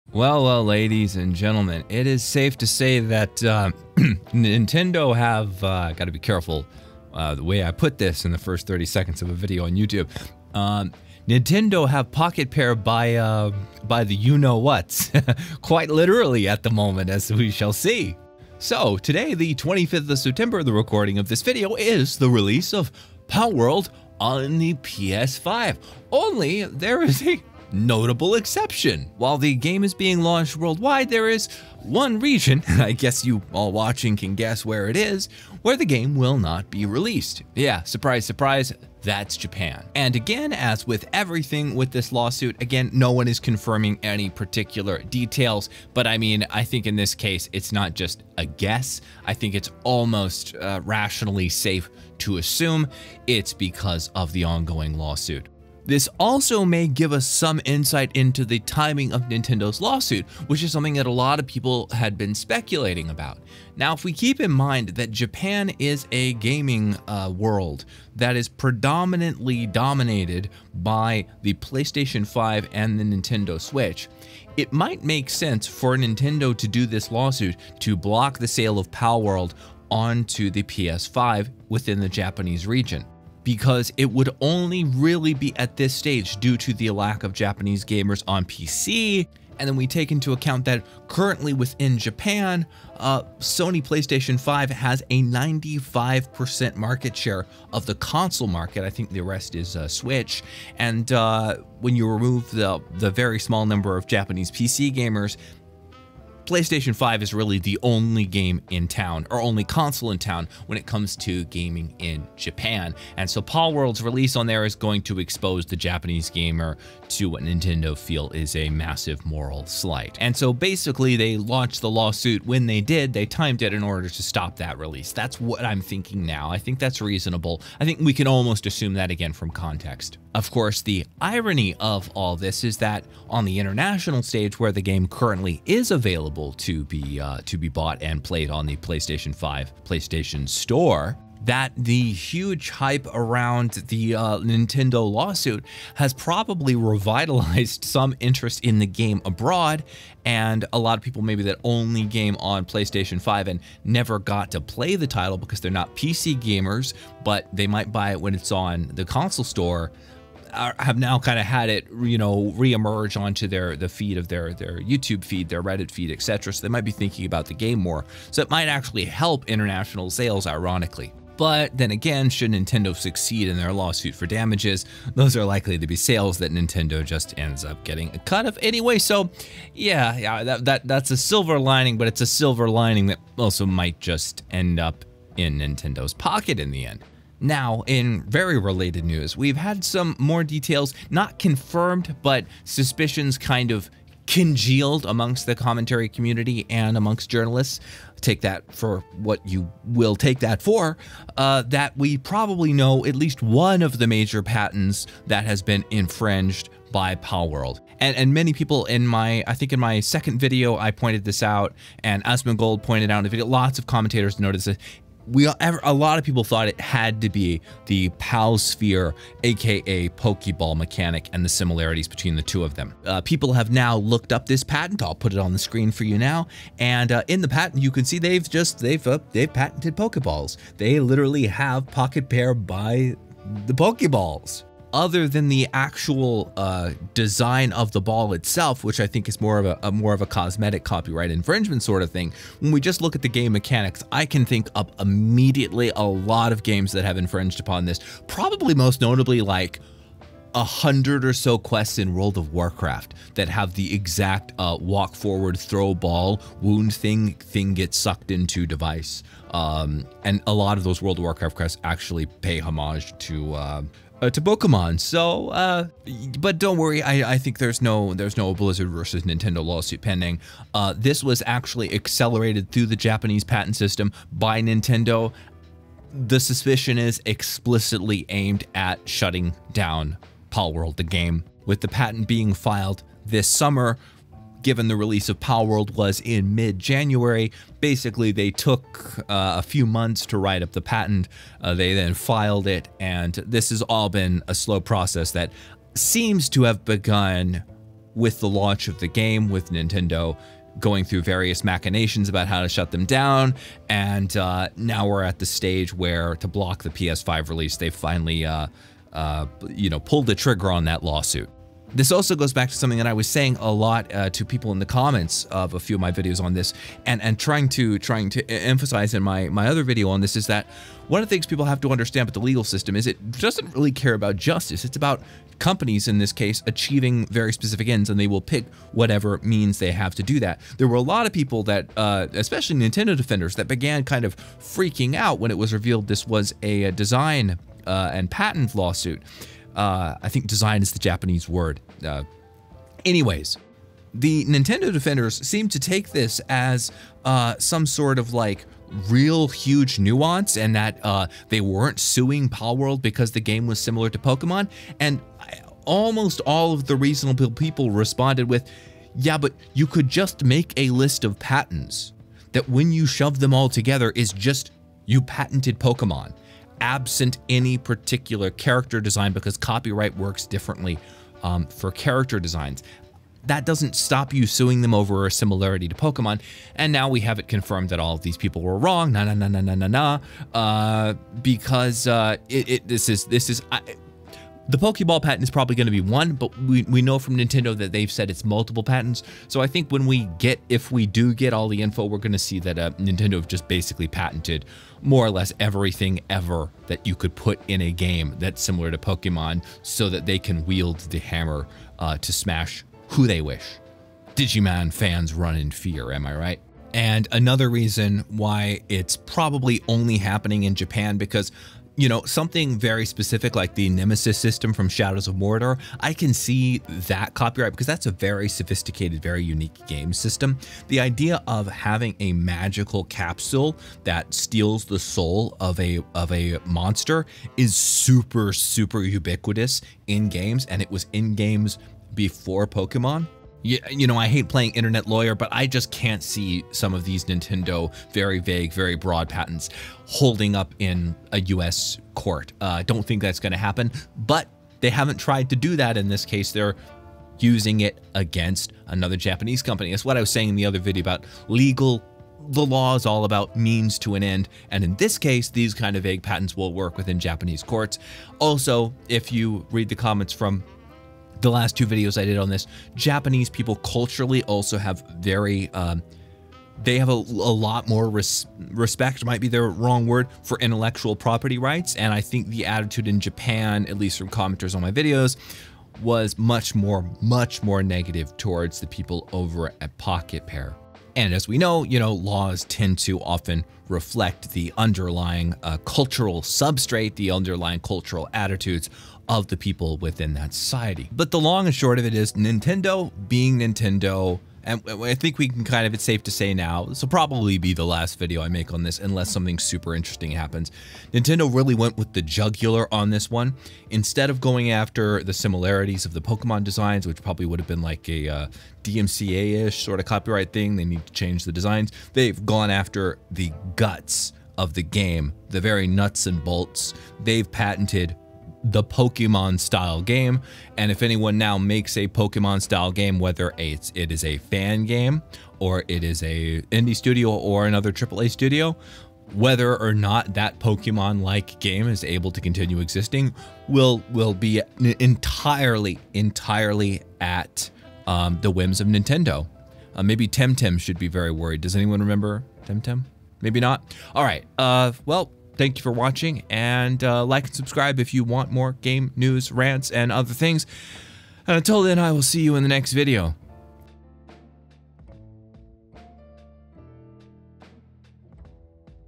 Nintendo Have Palworld Devs by The Pokeballs... As PS5 Release Blocked
The saga continues as we we know now probably why Nintendo filed the lawsuit when they did.